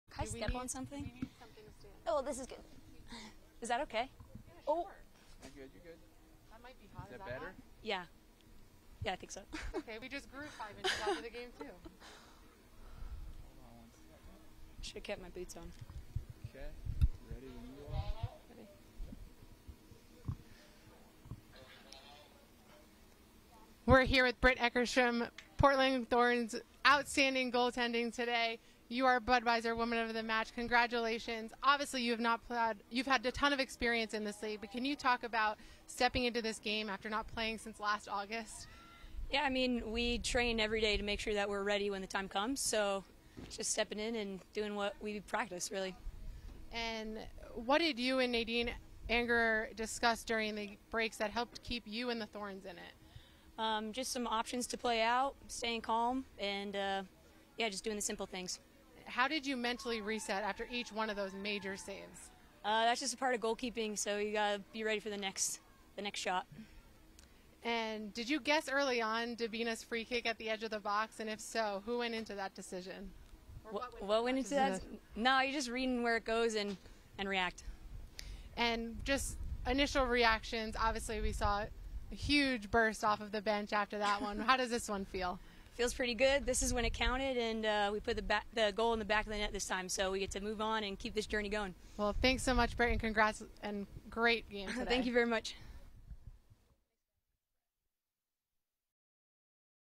Britt Eckerstrom - Budweiser Woman of The Match
Britt's postgame interview after a superhuman performance against the Courage.
BAONPDX | July 17, 2020